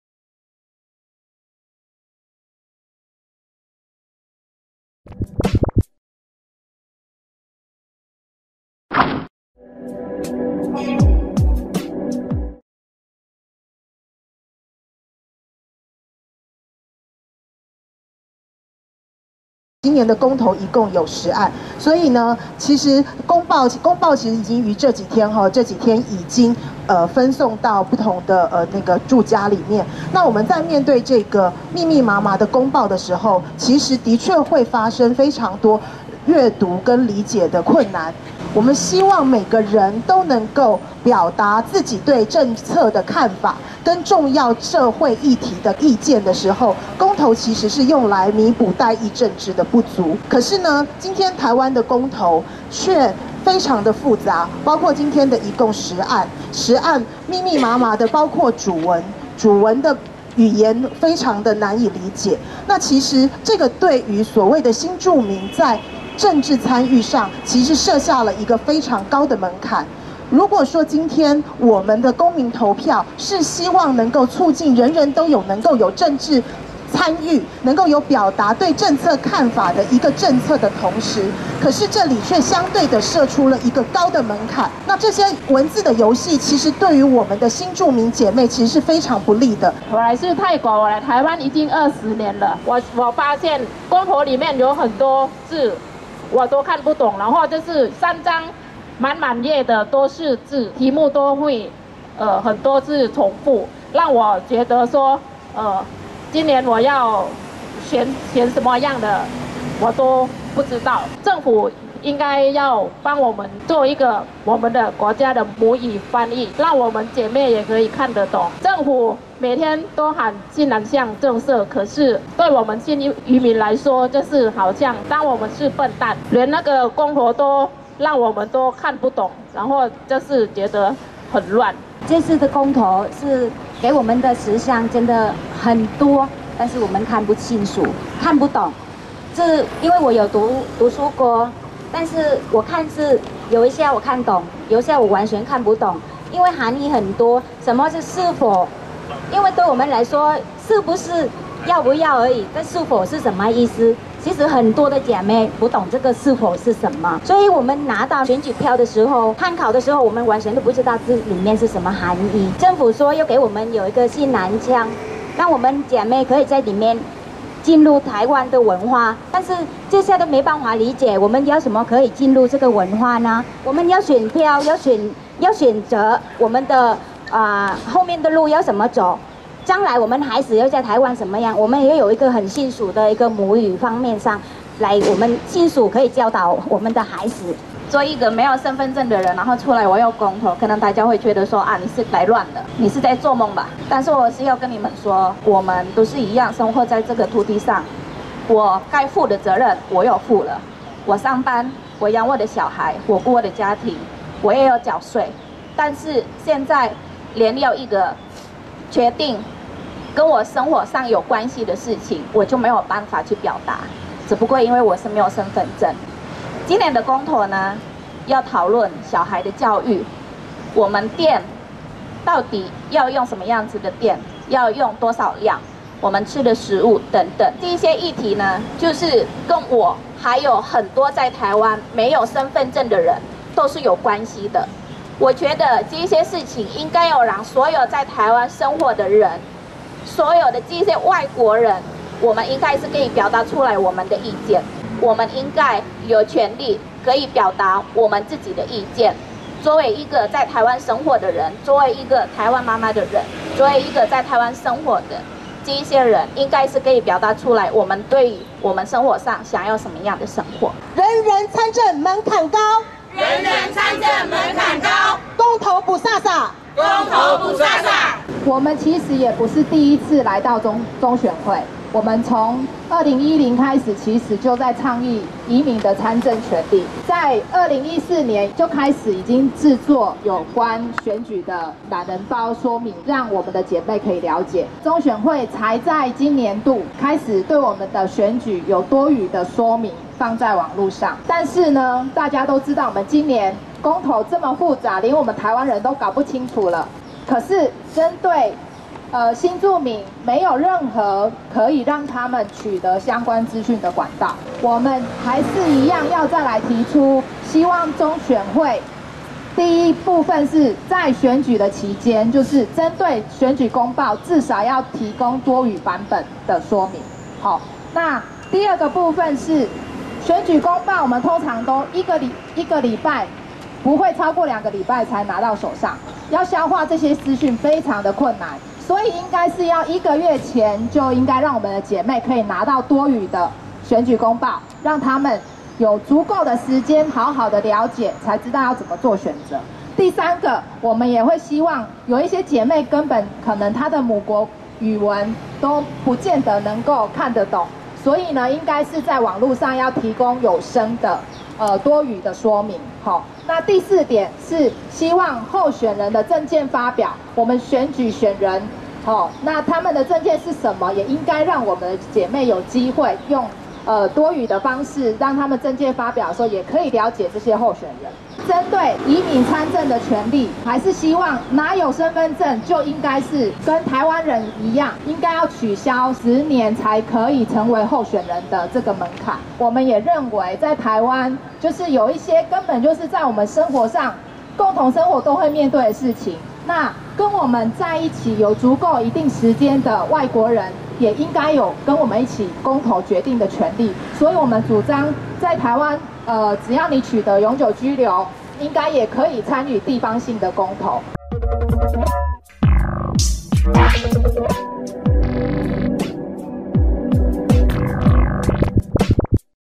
2018.11.16 讓投票更簡單 新住民要多語選舉公報
讓投票更簡單　新住民要多語選舉公報

距離九合一選舉和公投還有8天，台灣國際家庭互助協會、天主教嘉祿國際移民組織台灣分會、白刷刷黑戶人權行動聯盟等團體，今（11/16）天前往中央選舉委員會陳情，希望中選會考量新住民的處境，提供多國語言版本的選舉公報，保障新住民投票權益。國際家協更為此率先自製了越南文、印尼文、泰國文和英文等四國語文的公投懶人包，協助新住民理解內容。